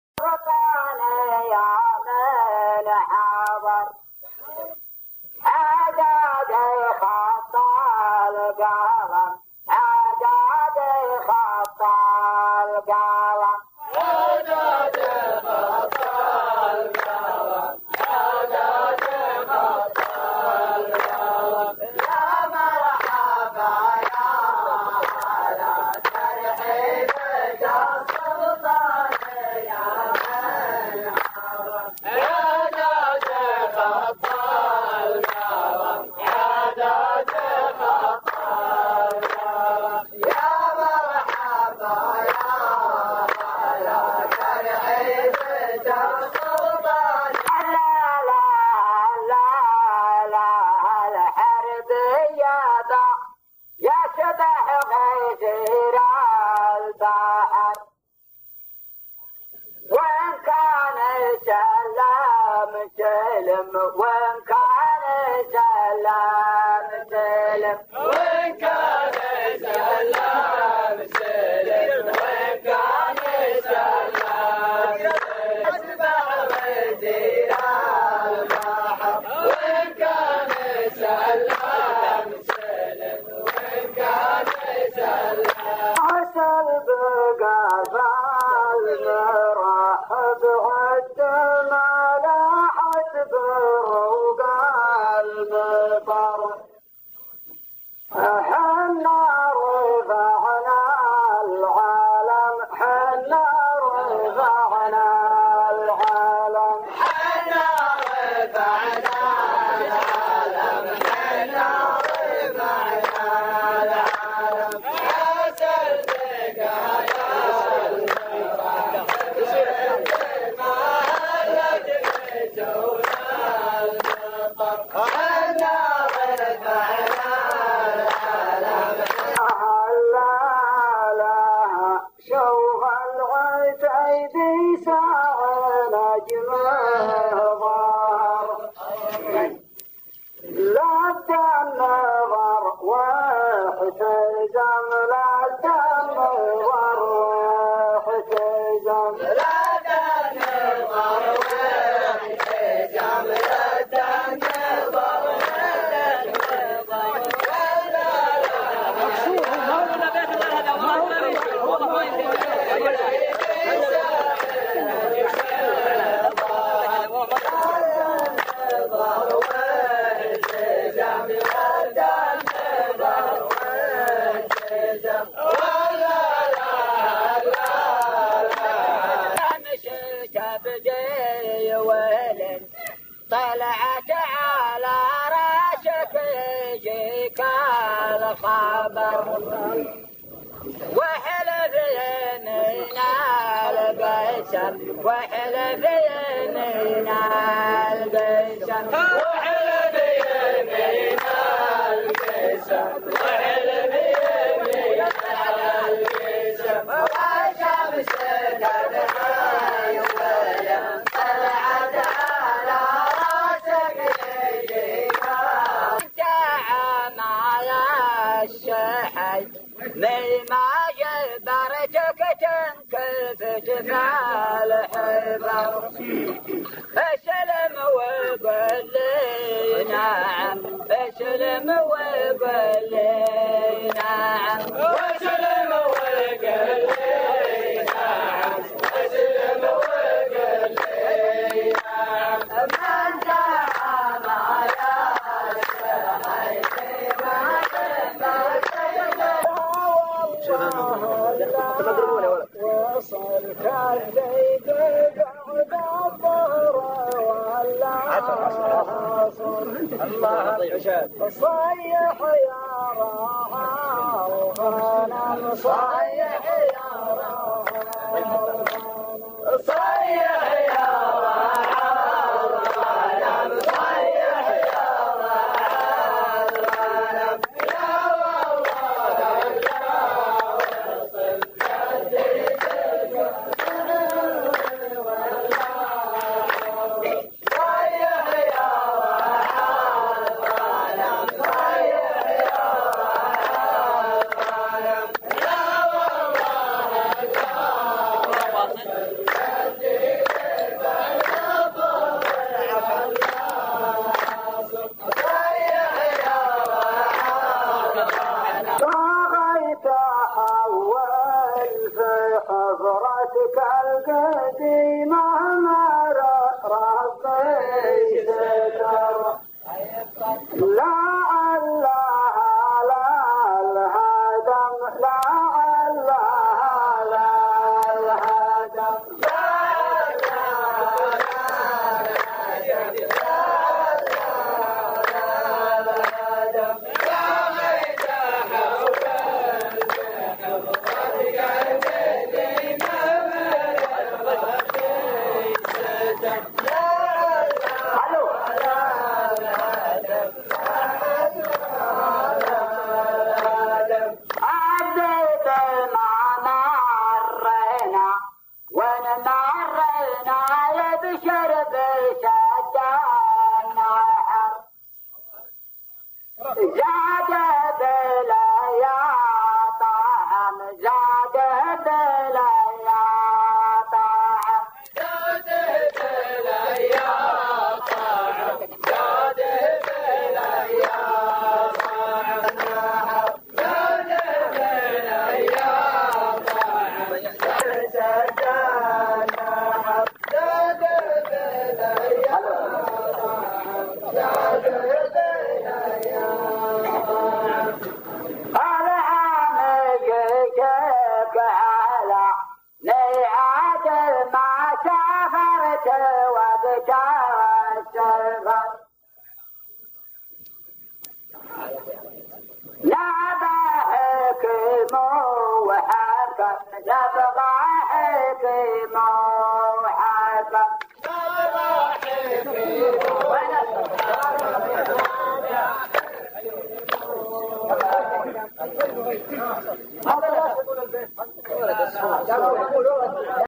ضلع طويق الشاعر صياف الحربي مع الشاعر جار الله السواط (بغيت احول في حفرتك القديمة مير ربي ستر)
من تسجيلات منتديات فلاح بن صياف هذه المحاورة